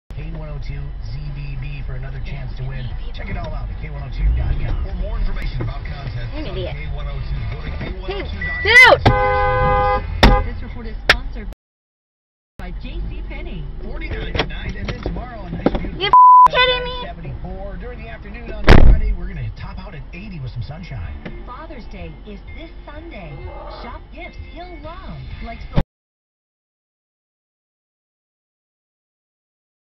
Can You Believe This Hit and Run? | Captured on the Momento M6
Minnesota residents! Be on the lookout for this driver.
Did you know 682,000 hit and runs occur in the US each year? Of those 682,00, only 8% are solved. Hopefully, this Momento M6 footage will help the authorities quickly track down this driver!